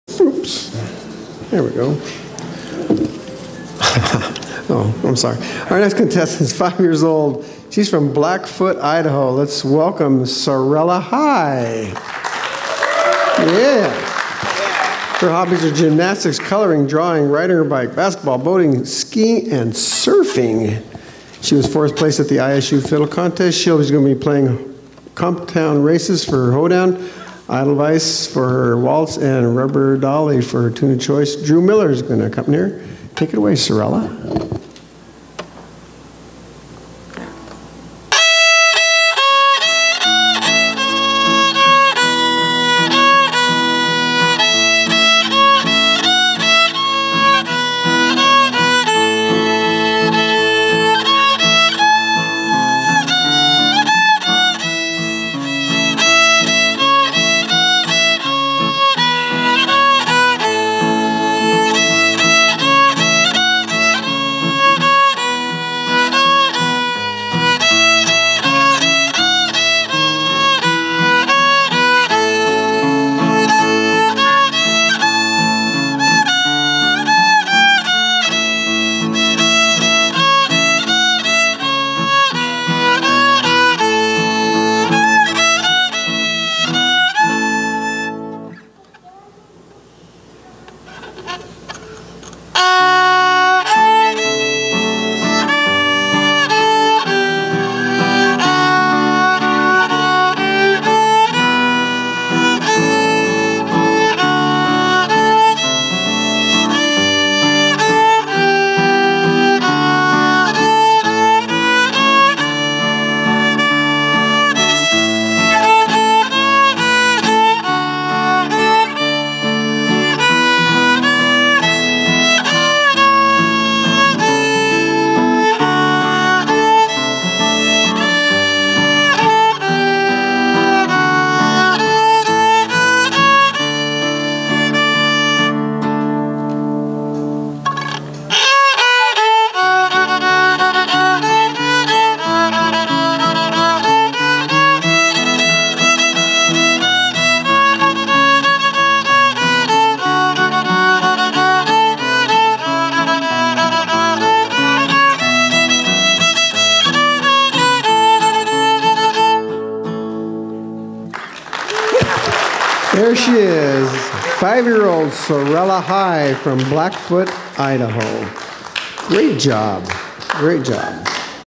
2017-06-19 SF1 Sorella High - Weiser Fiddle Contest 2017
Small Fry Fiddle Division - Round 1
National Oldtime Fiddlers Contest & Festival  2017